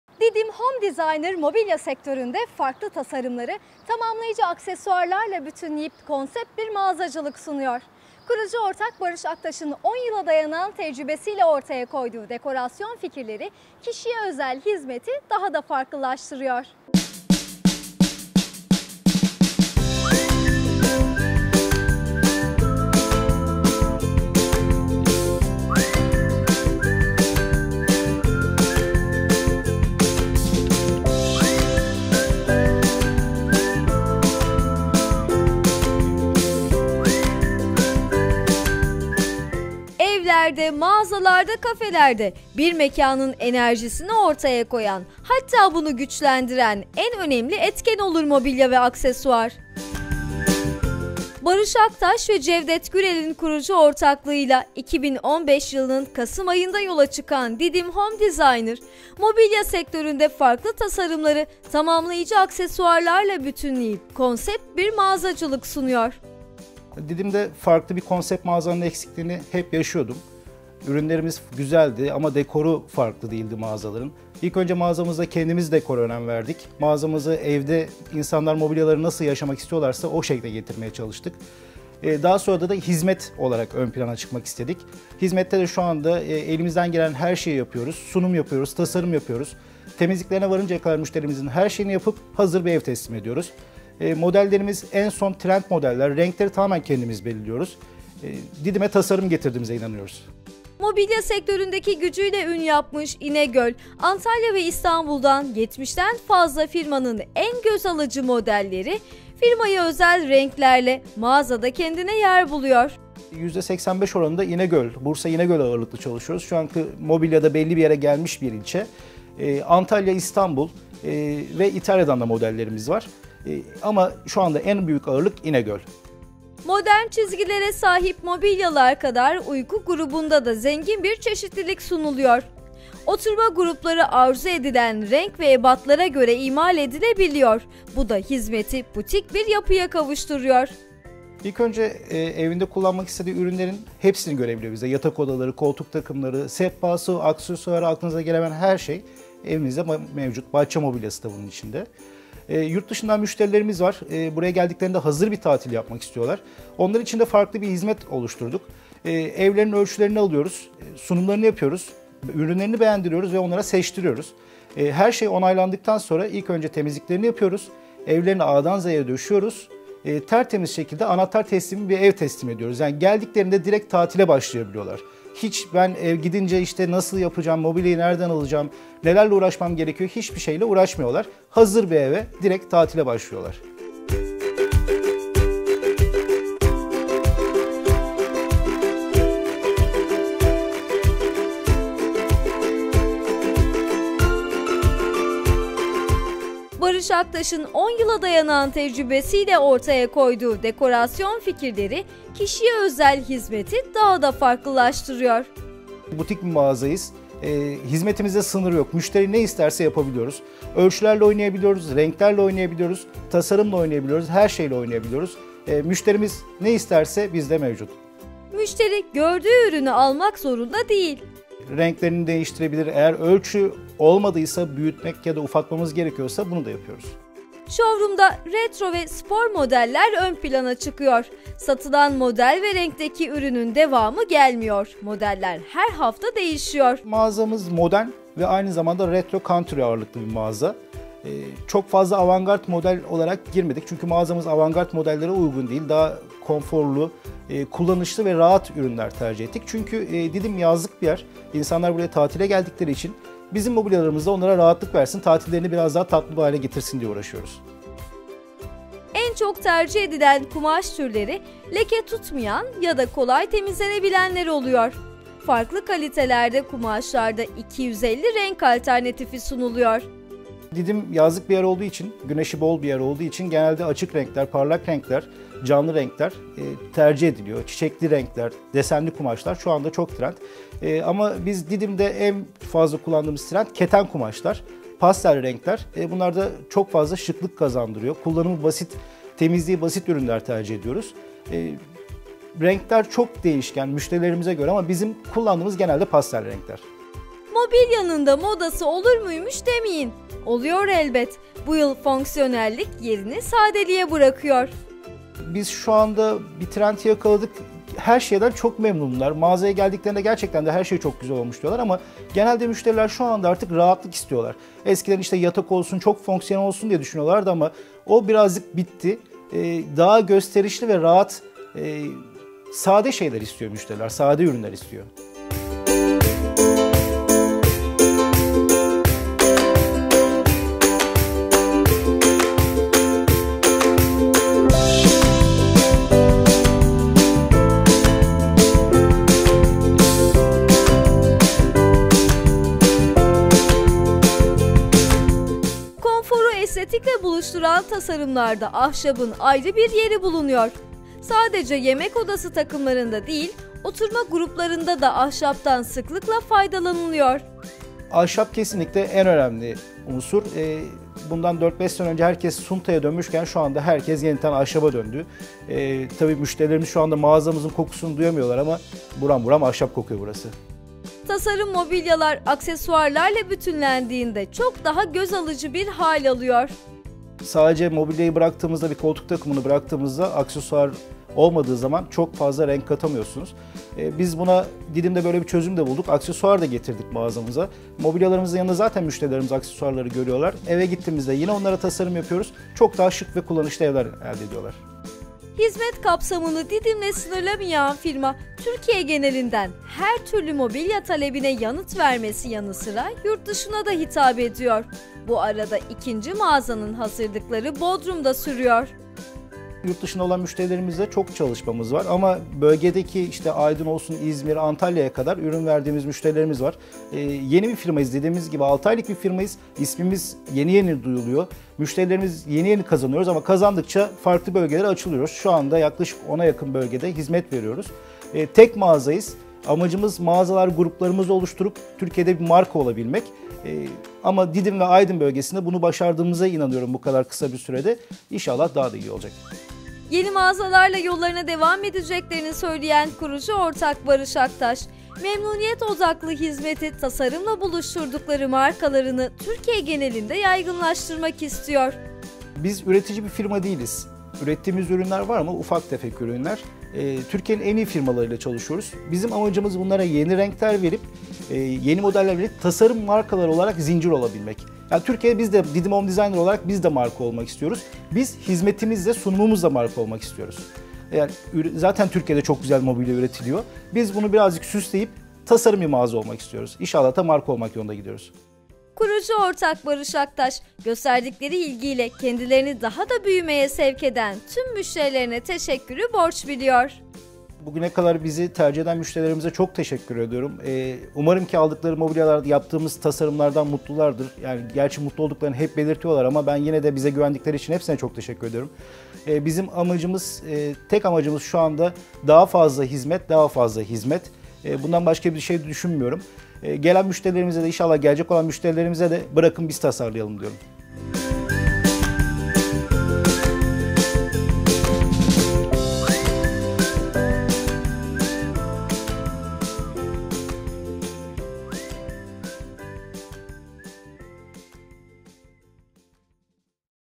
DİDİM HOME DESİGNER
Didim Home Designer, mobilya sektöründe farklı tasarımları, tamamlayıcı aksesuarlarla bütünleyip konsept bir mağazacılık sunuyor. Kurucu ortak Barış Aktaş'ın 10 yıla dayanan tecrübesi ile ortaya koyduğu dekorasyon fikirleri kişiye özel hizmeti daha da farklılaştırıyor.